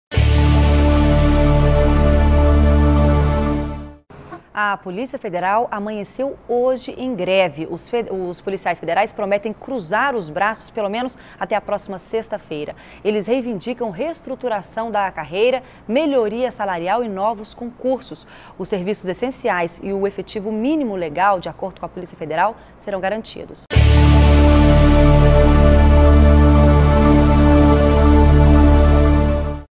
Polícia Federal entra em greve a partir de hoje
A Polícia Federal entra em greve até a próxima 6a. Feira por melhores salários, reestruturação da carreira e novos concursos.